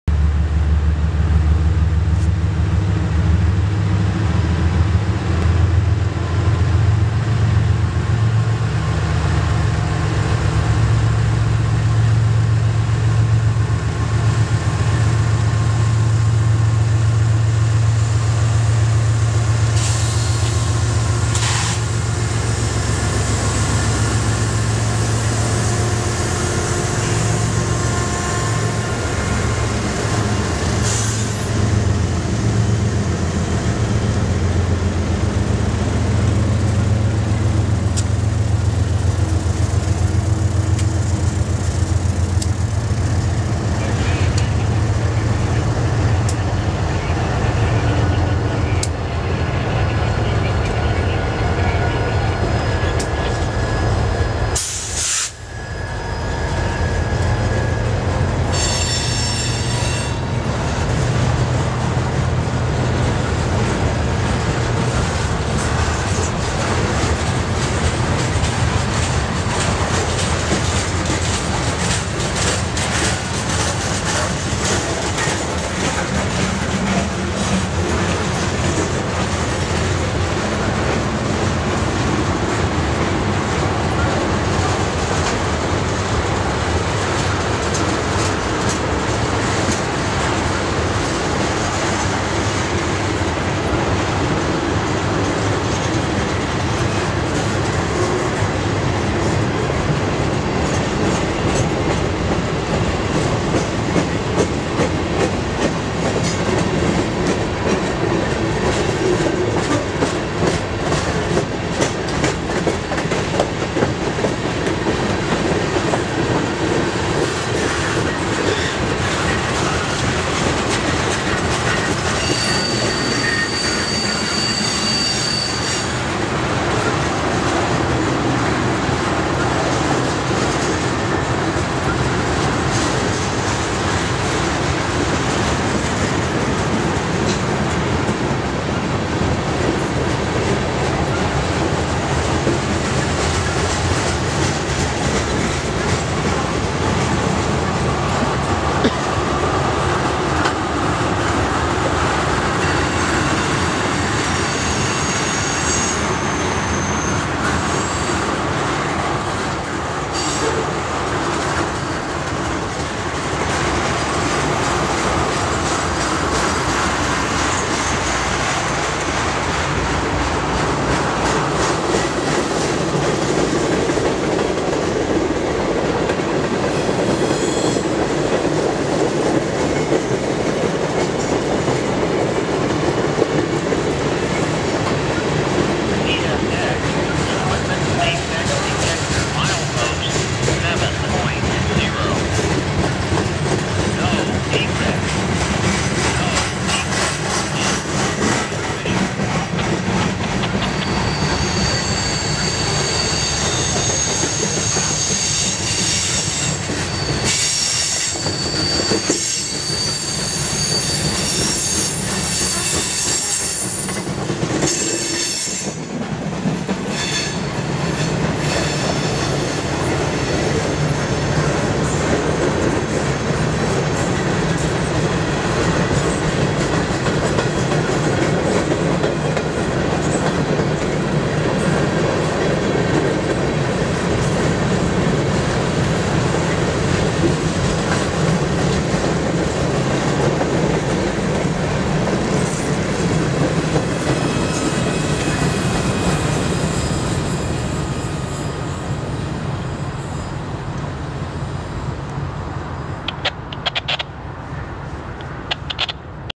CSX Q-411 EB On The Old Main Line
CSX Q-411 crawls off the curve, slowly accelerating on the Old Main Line heading EB toward Baltimore. ES44DC 5424 is the leader, 2nd unit is ES44DC 5255, 3rd unit is SD80MAC 4602, 4th unit is CW60AC 656, 5th unit is GP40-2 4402, and the 6th unit is SD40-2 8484. Well guys hence again why I get easily fed up with CSX, here I caught the not at all common SD80MAC for the Baltimore area as one of the trailing units and come on? How awesome would it have been to see 4602 leading the parade??!! No but of course CSX stuck 2 damn GEVOs up front, insulting (in my opinion) the 80MAC and the other nice power that followed. Just yet another chapter in the clueless novel of CSX. Anyway, it turned out that 4602 led Q-438 on December 31st, then sat at Landsdowne for the New Year's shut down on CSX.